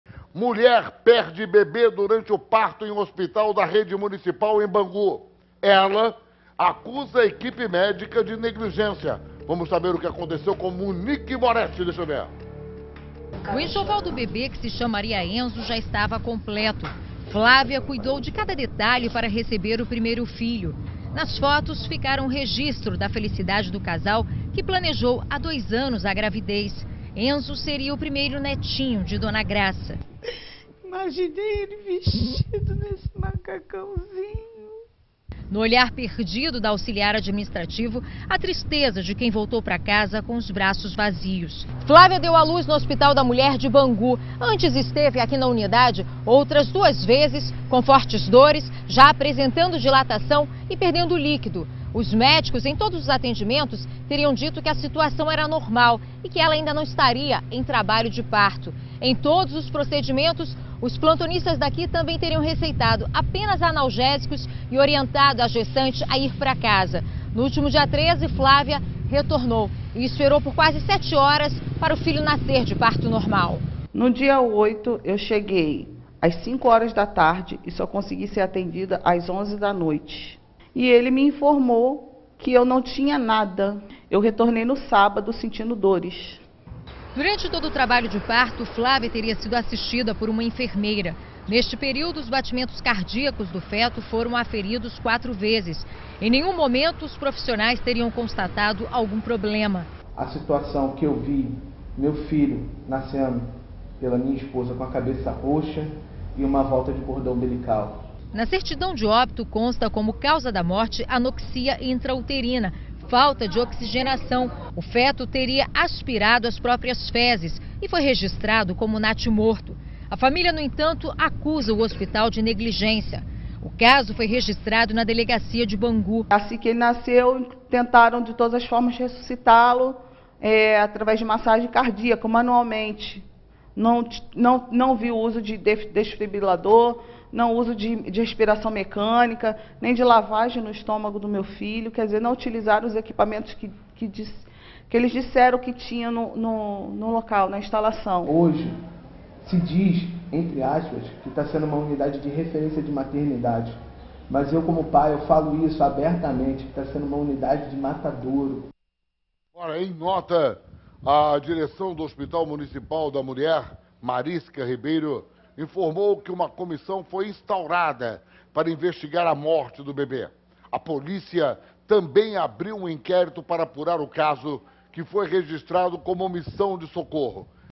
Mulher perde bebê durante parto em Bangu (RJ)
A mulher acusa o hospital da rede municipal, em Bangu, na zona oeste do Rio de Janeiro de negligência. Ela acusa a equipe médica de negligência.